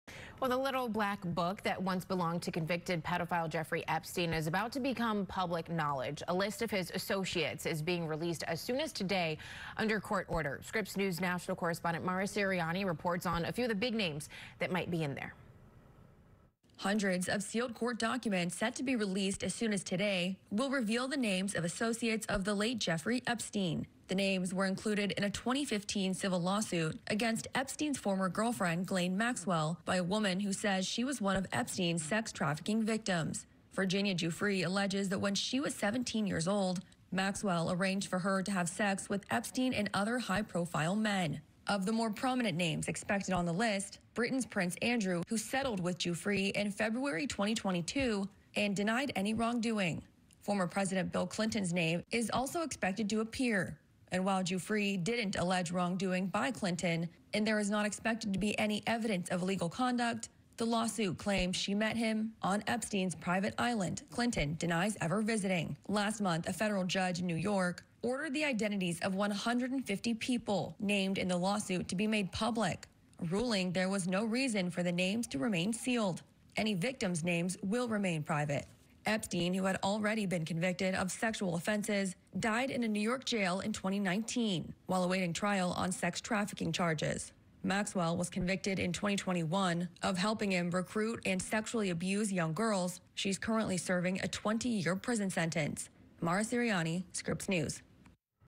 List of Jeffrey Epstein's associates soon to be made public
The names of more than 150 people tied to Jeffrey Epstein will soon be made public following a New York judge's order to unseal a trove of court documents currently identifying them as "J. Doe."

The documents hail from a civil lawsuit in which Virginia Giuffre accused Epstein's romantic partner, Ghislaine Maxwell, of facilitating sexual abuse against her starting when she was 16. The lawsuit was settled two years later, in 2017, and the court soon after granted the Miami Herald's motion to unseal the case's documents.